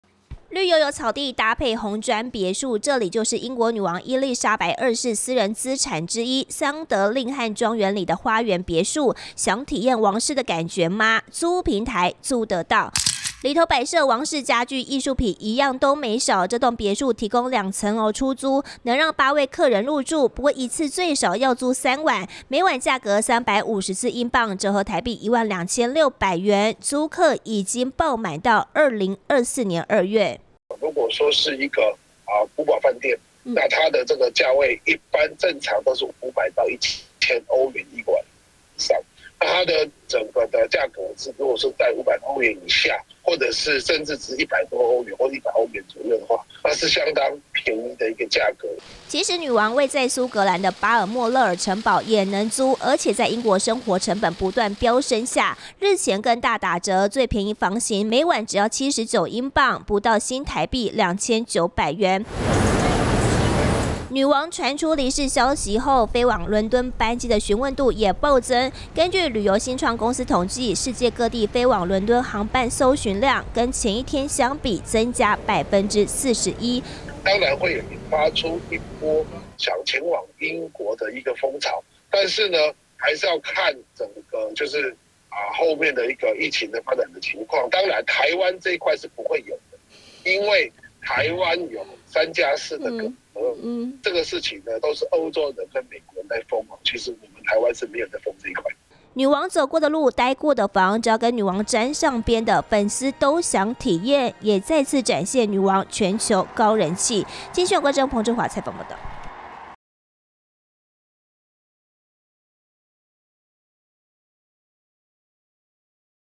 體驗王室度假風 「英國女王別墅」滿租到2024年｜鏡新聞
現在有關女王的一切大小事，都被翻出來討論，從他說過的話，做過的事，甚至連他的穿著品味打扮，都掀起風潮。最近租屋平台還釋出了女王住宅，「桑德令罕莊園」裡的「花園別墅」，每晚要價1萬2600元台幣，現在訂，要等2024年。